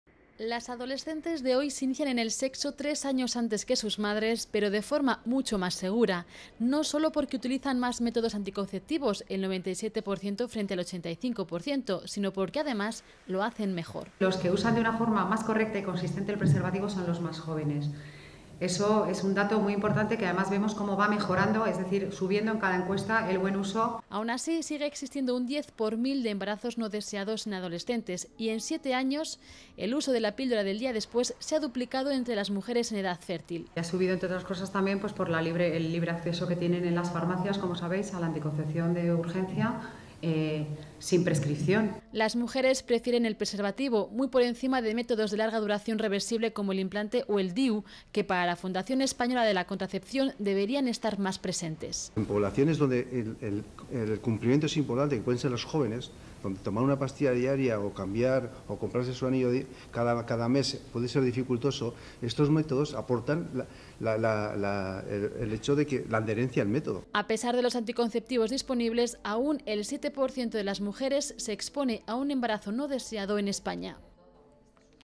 Adolescentes más precoces pero mejor informadas
Según la Encuesta Nacional sobre Anticoncepción, titulada 'Hábitos de la población femenina en relación al uso de métodos anticonceptivos', encargada por el Observatorio de Salud Sexual y Reproductiva de la SEC.